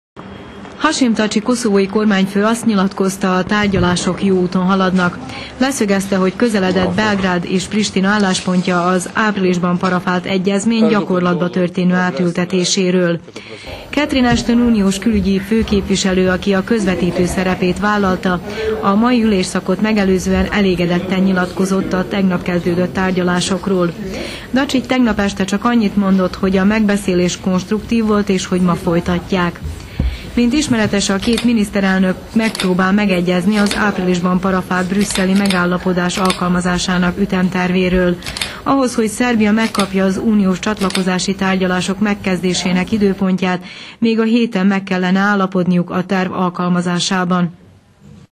A brüsszeli tárgyalás második napja
Négy órás szünet után délután folytatódott Belgrád és Prishtina párbeszéde Brüsszelben. Előbb Hashim Thaci koszovói kormányfővel, majd Ivica Dačić szerbiai miniszterelnökkel folytatott bilaterális tárgyalásokat Catherine Ashton, az EU külügyi főképviselője. A Koszovó és Szerbia kapcsoalatainak normalizálását célzó brüsszeli egyezmény végrehajtási tervéről az esti órákban tartanak háromoldalú megbeszélést.